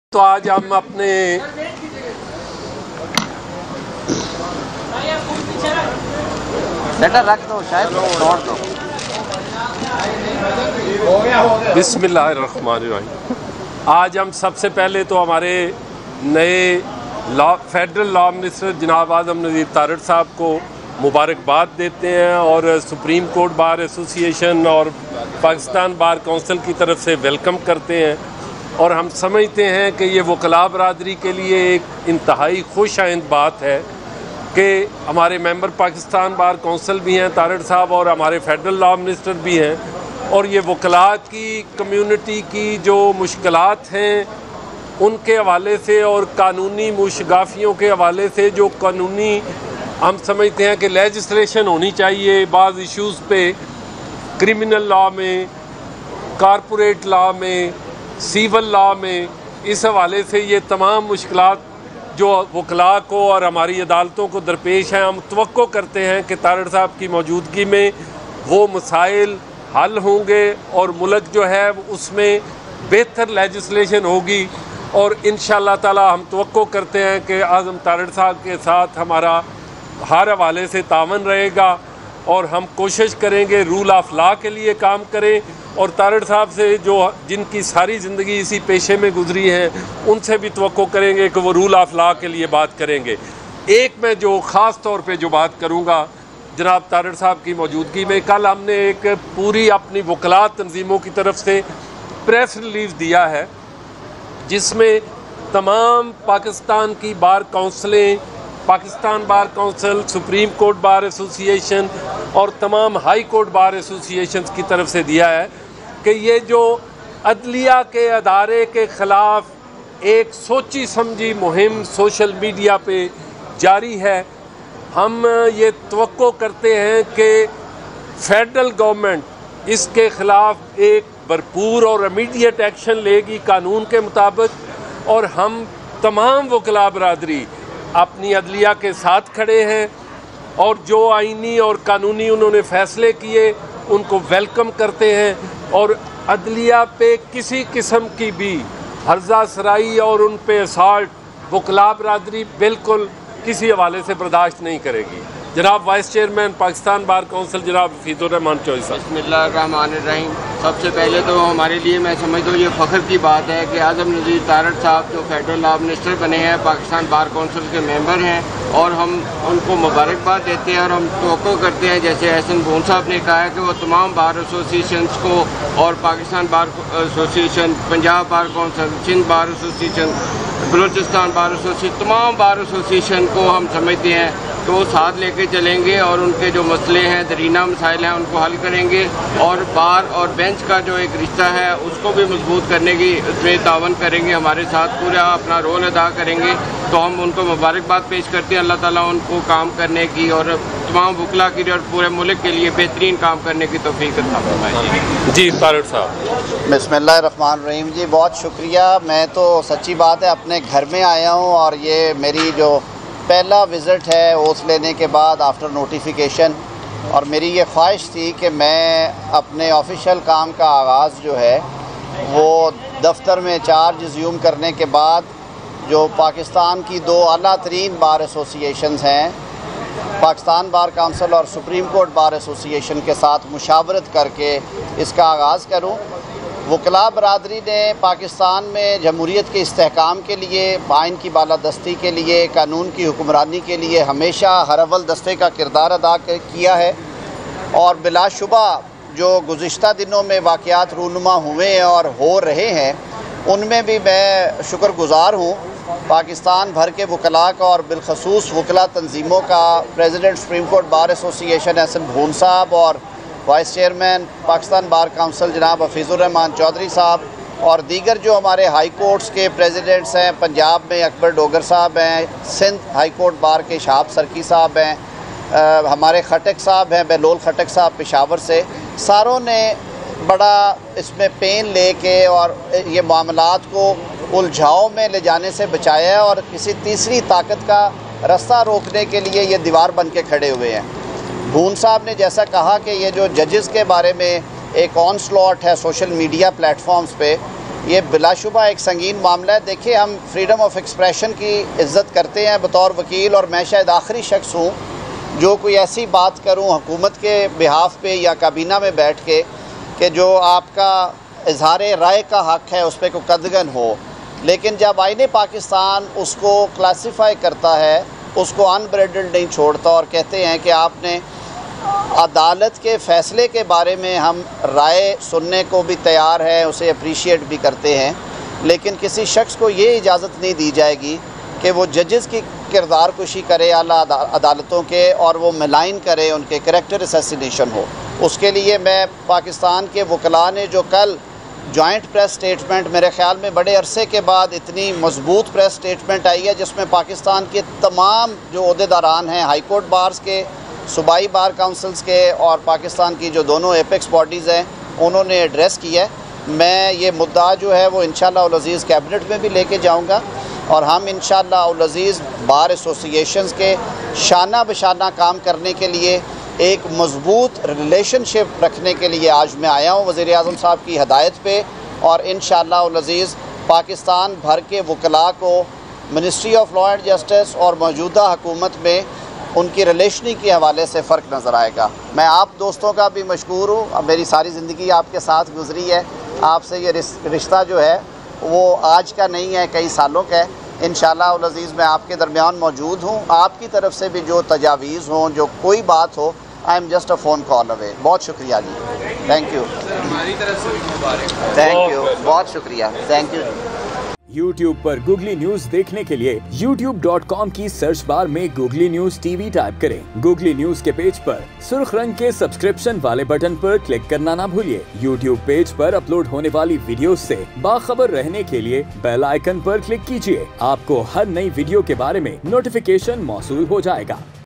Naye Law Minister Azam Nazir Tarar Ka Adalya Mukhalif Muhim Counter Karnay Ka Eilan | Googly News TV
Naye Law Minister Azam Nazir Tarar Ka Adalya Mukhalif Muhim Counter Karnay Ka Eilan